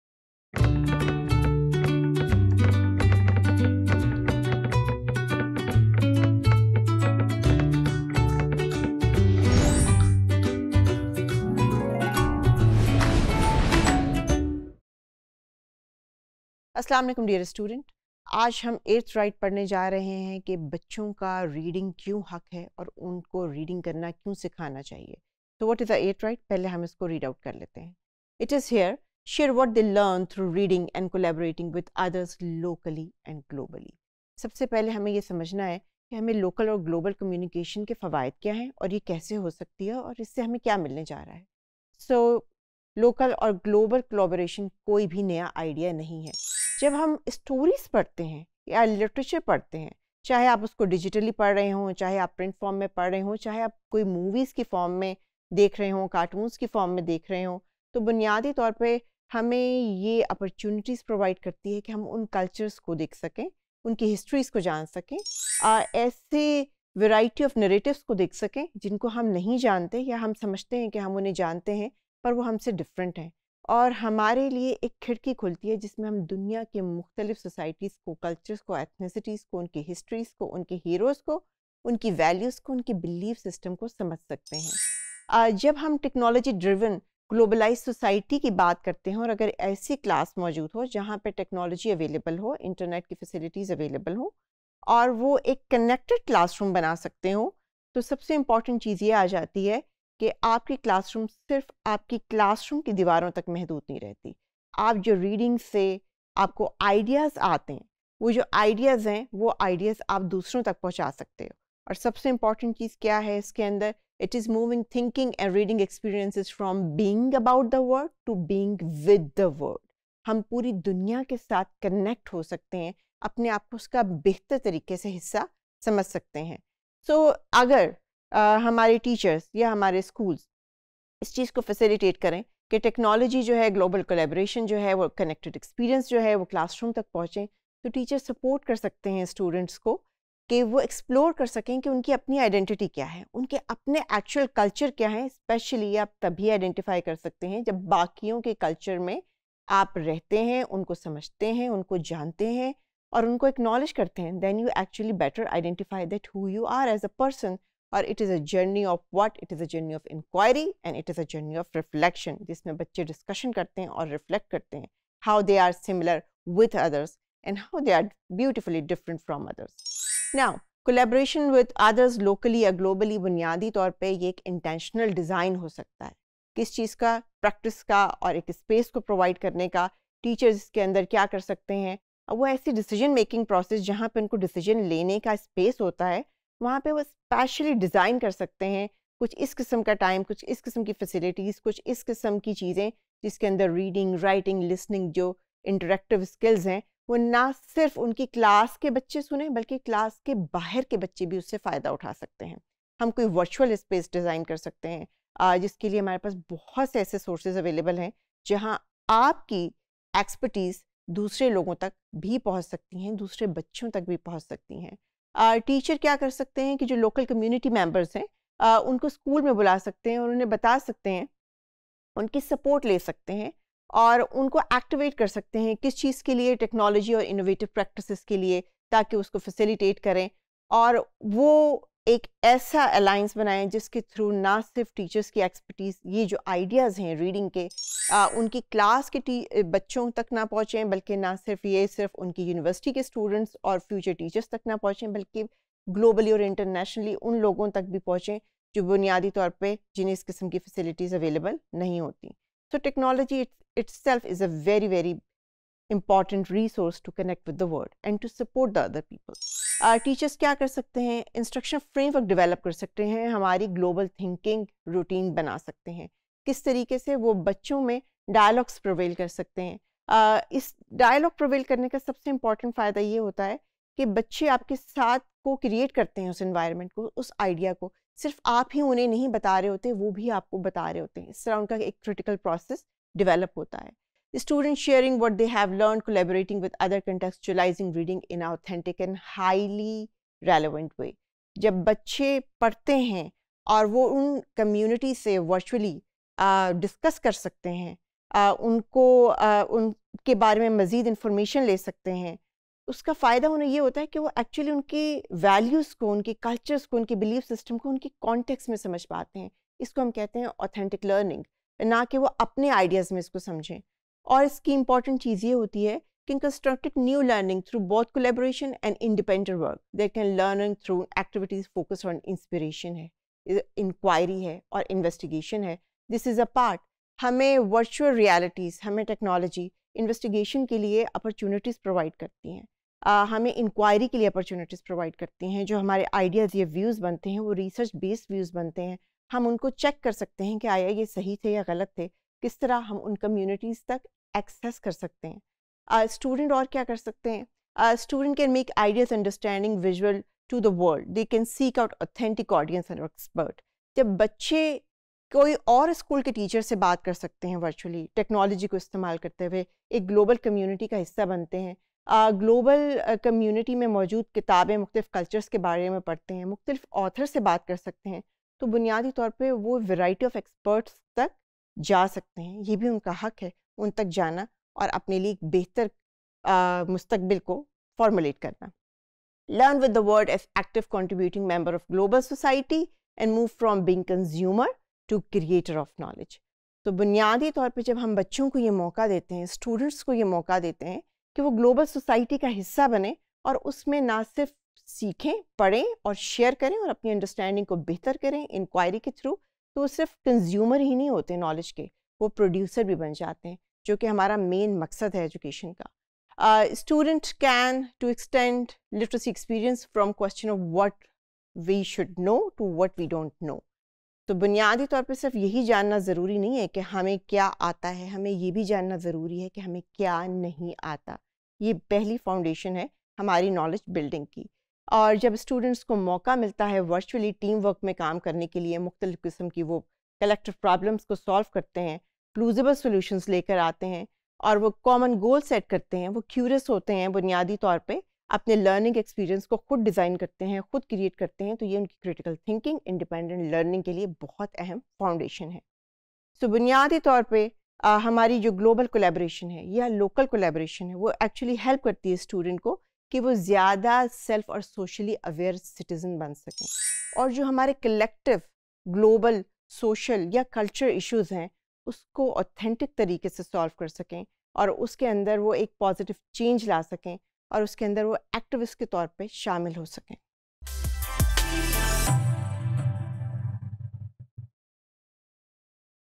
Eighth Right: Share What They Learn | Children's Literature & Educational Games | ECE203_Topic046
Topic046: Eighth Right - Share What They Learn Through Reading by Collaborating with Others Locally & Globally,